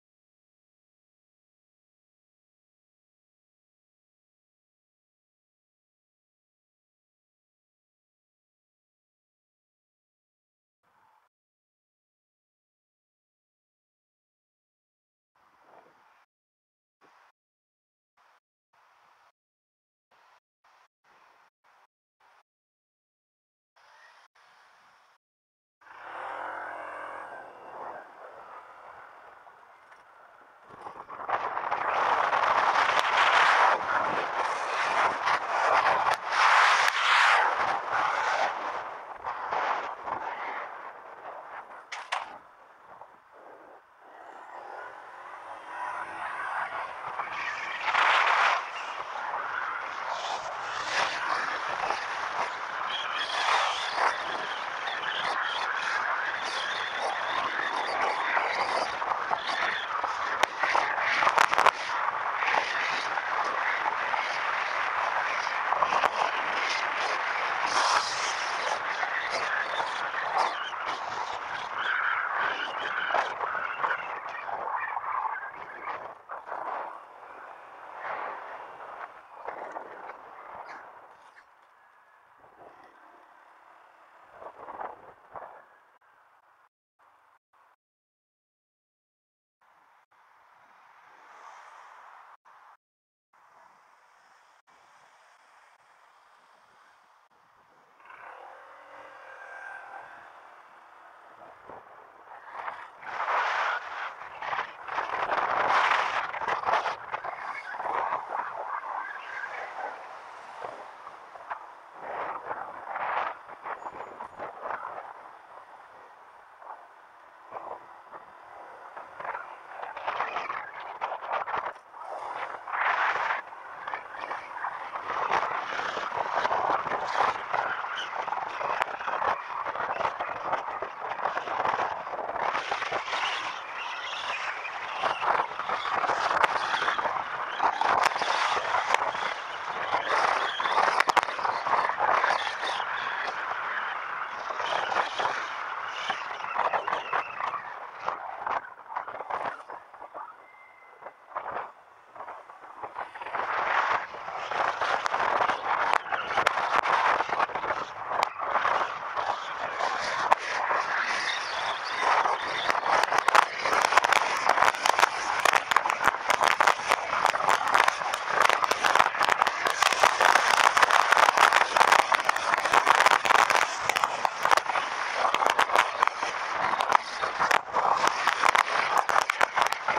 to Busan, South Korea, nature bike riding in the city
South Korea Busan City Drive in a hidden area in Busan, South Korea
Haeundae, Gwangan-ri, countryside, city center, and tourist destination drive Actual Driving ActionCamRecording @ 04/20/2023